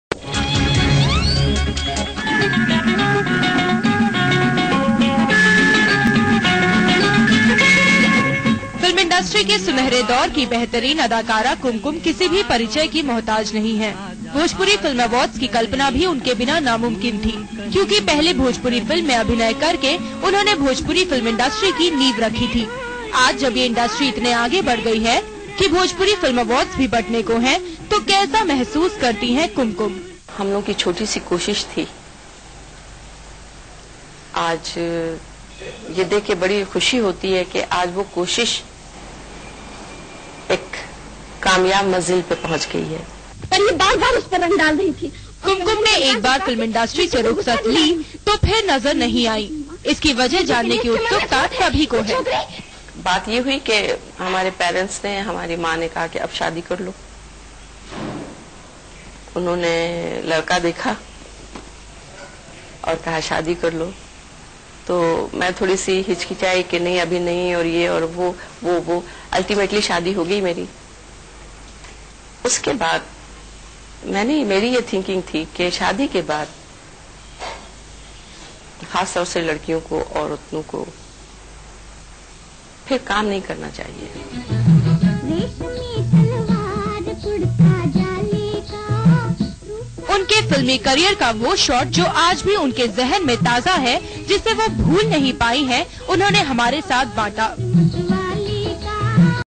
1stBhojpuri Film Awards On DD News
1st Bhojpuri Film Awards, Gust Dilip Kumar, Saira Banu, Govinda and others hindi & bhojpuri film stars